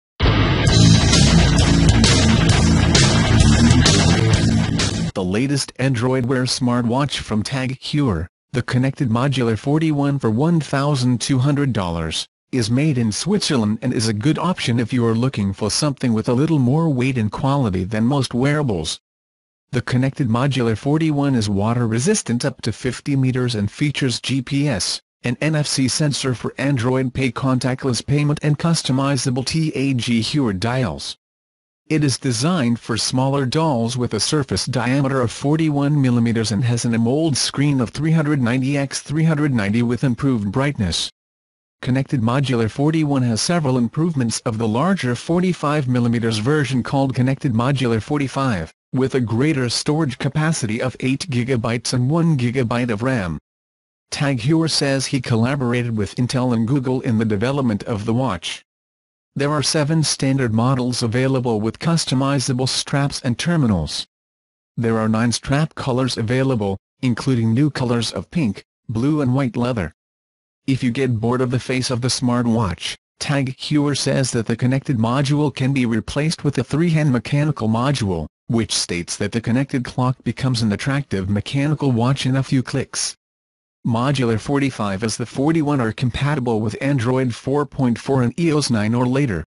Latest Android Wear Tag Heuer new watch features dials straps and customizable terminals
The latest Android Wear smart watch from Tag Heuer, the Connected Modular 41 for $ 1,200, is made in Switzerland and is a good option if you are looking for something with a little more weight and quality than most wearables. The Connected Modular 41 is water resistant up to 50 meters and features GPS, an NFC sensor for Android Pay contactless payment and customizable TAG Heuer dials. It is designed for smaller dolls with a surface diameter of 41mm and has an AMOLED screen of 390 x 390 with improved brightness. Connected Modular 41 has several improvements of the larger 45 mm version called Connected Modular 45, with a greater storage capacity of 8 GB and 1 GB of RAM.

Tag Heuer says he collaborated with Intel and Google in the development of the watch. There are seven standard models available with customizable straps and terminals. There are nine strap colors available, including new colors of pink, blue and white leather. If you get bored of the face of the smart watch, Tag Heuer says that the connected module can be replaced with a three-hand mechanical module, which states that "the connected clock becomes an attractive mechanical watch in a few clicks. Modular 45 as the 41 are compatible with Android 4.4 and iOS 9 or later.